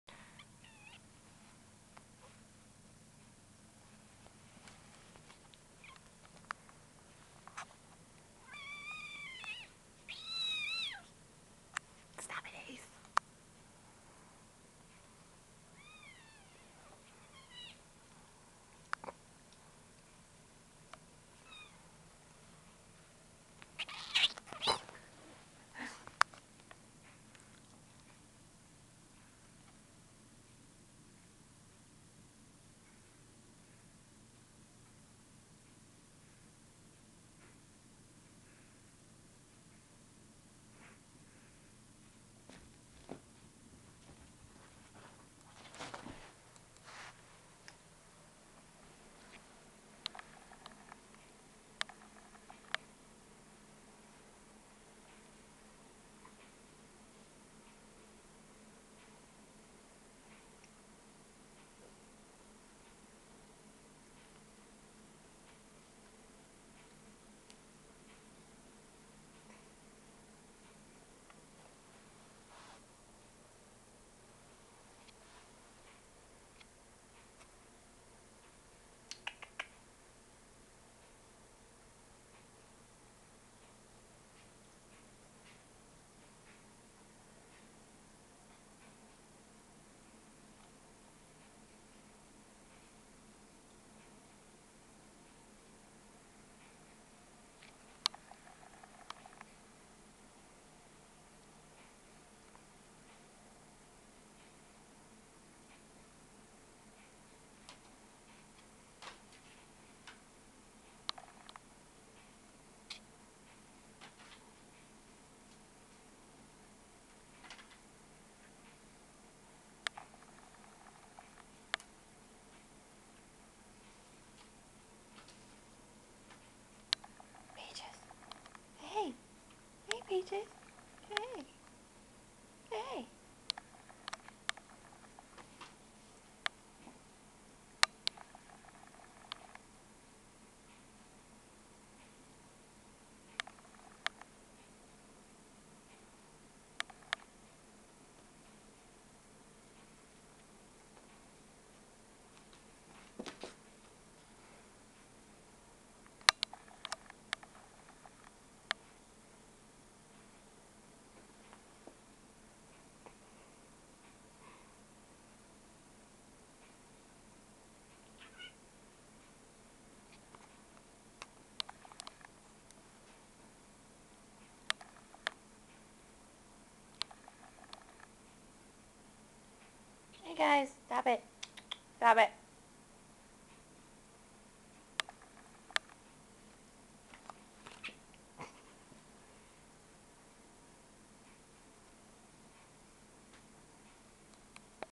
Kitties Playing
My two cats, Ace and Gary, enjoying a bit of play time.

2:40 for uber sneak attack.